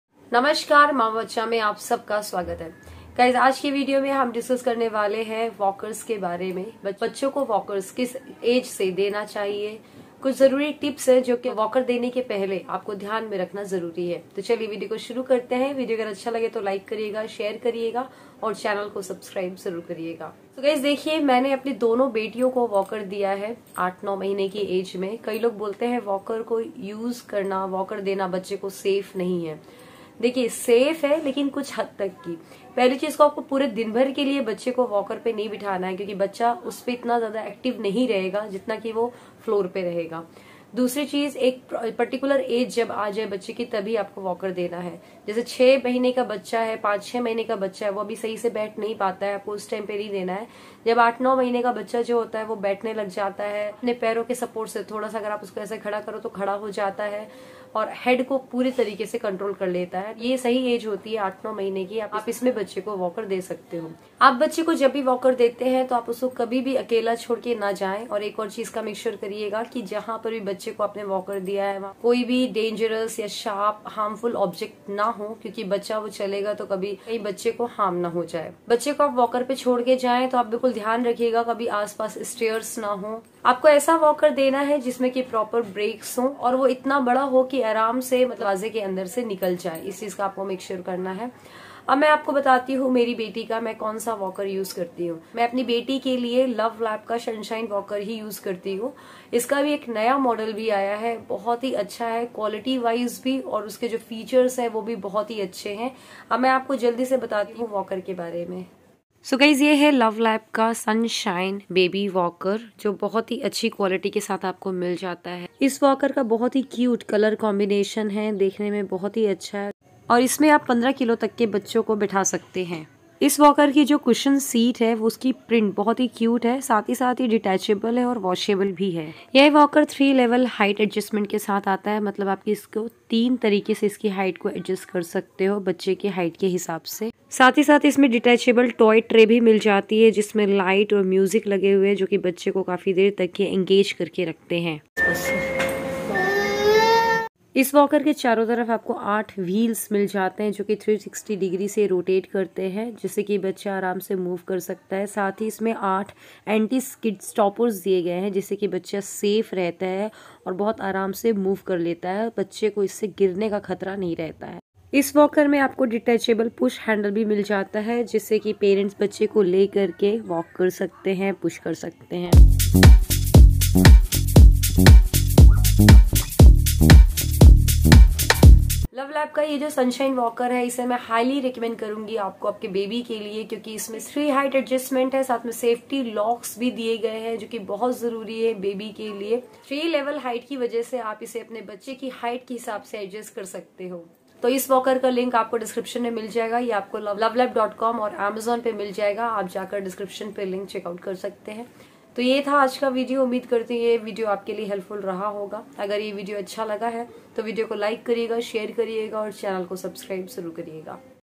मेरी बेटी का walker...Baby को walker कब से देना चाहिए.. walker देने के पहले जरूरी tips
LuvLap Walker